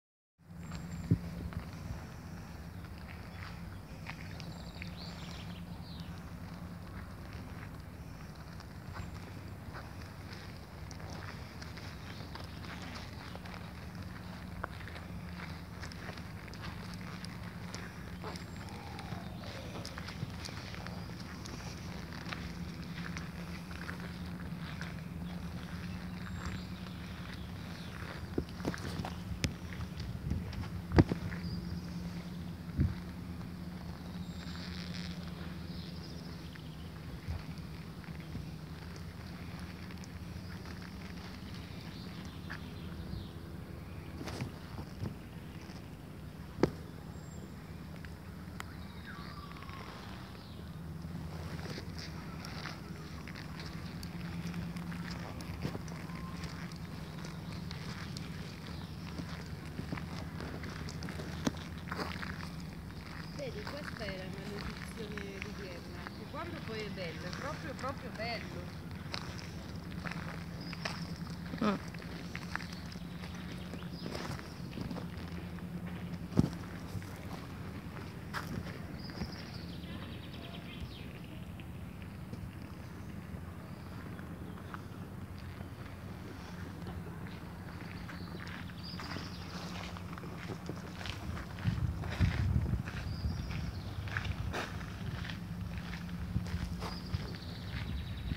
Vienna-Shoenbrunn 24 aprile 2017 Un'oasi imperiale....aperta al pubblico..🌳🌳🍃🍀🌹
Primavera a intervalli...oggi splendida giornata nel verde...